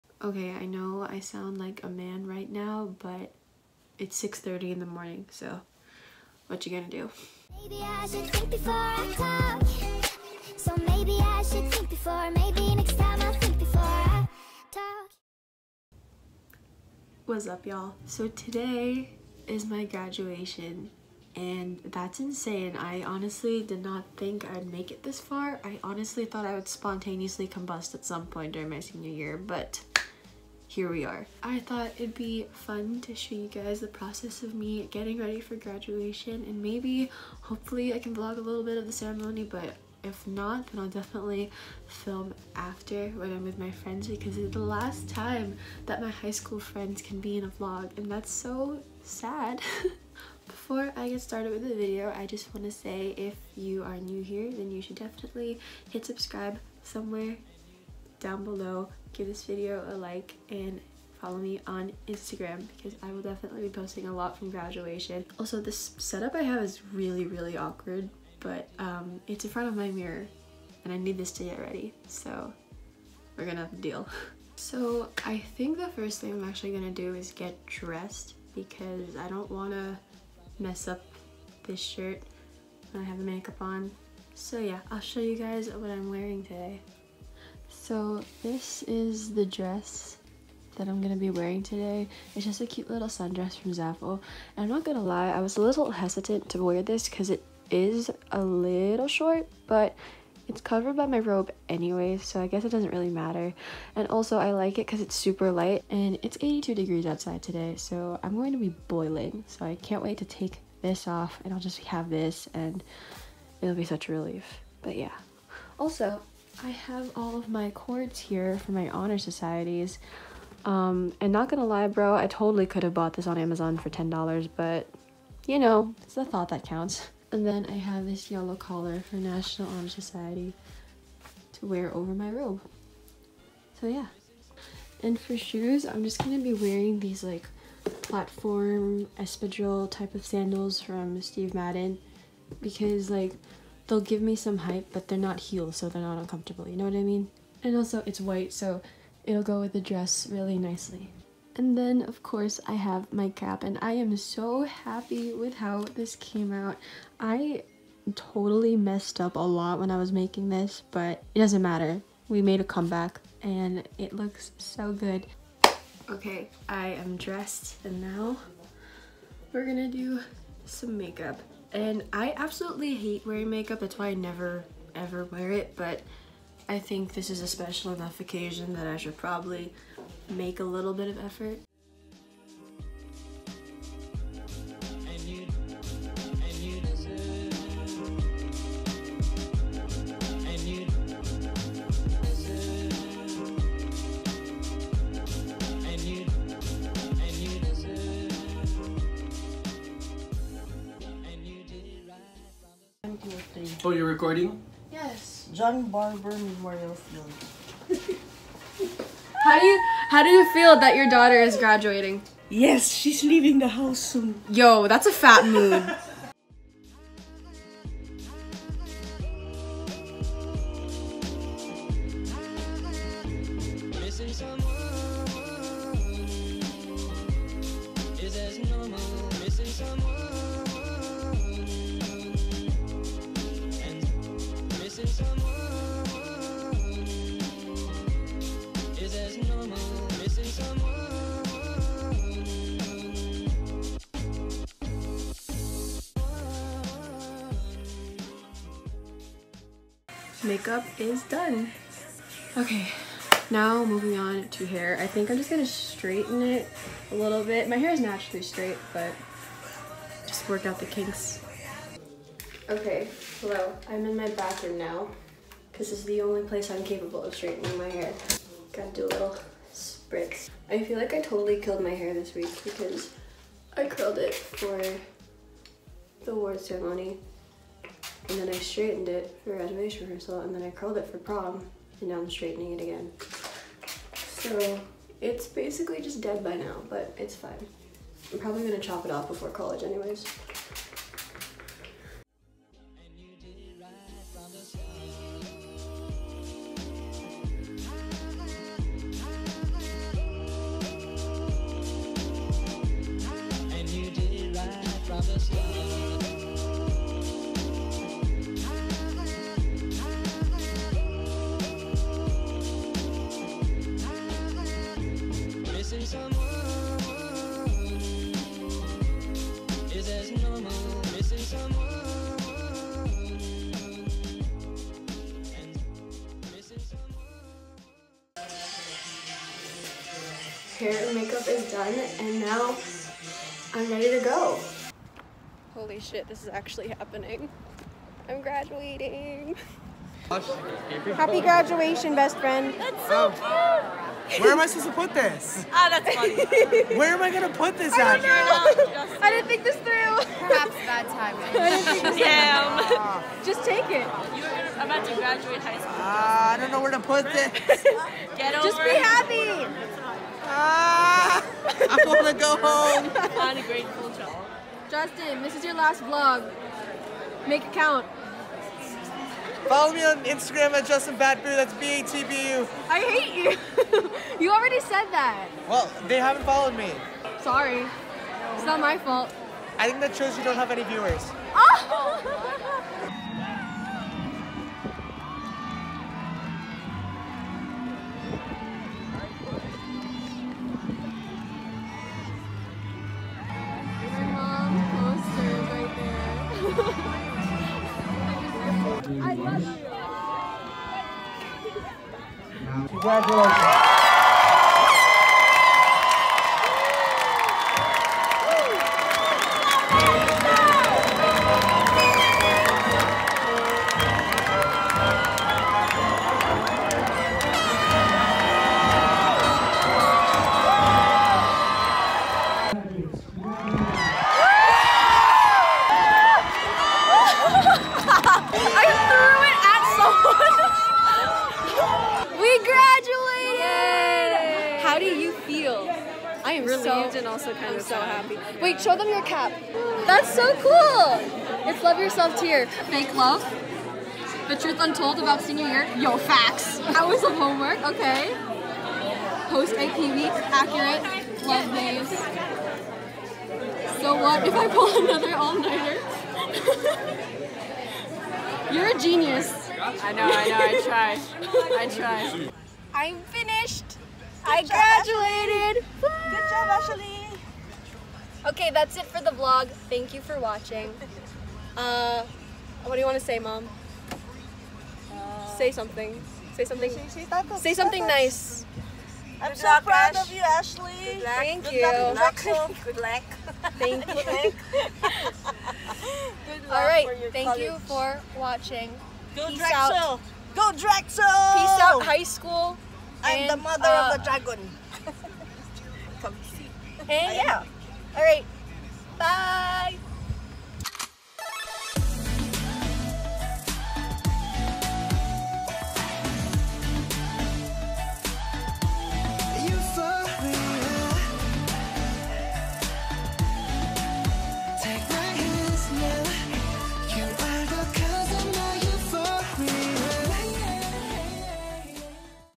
graduation grwm & vlog // Ashley Claire
sooo i graduated!! I can't believe how fast high school flew by but I'm ready to go to college and start this new chapter in my life. Thank you to everyone that made my high school experience memorable and good luck to the Class of 2019!